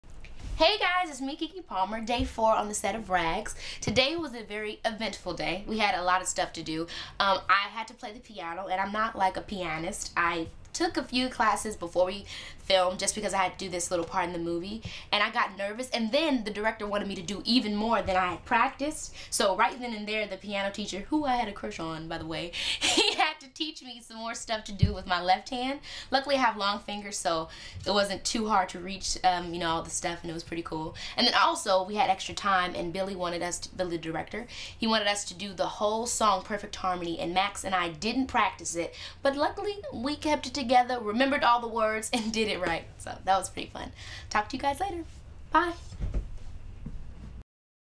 Day 4: Keke Palmer Diary on the set of Rags
Keke dishes on how she prepared with a piano instructor before filming a scene of Rags. Rags premieres May 28th at 8/7c on Nickelodeon!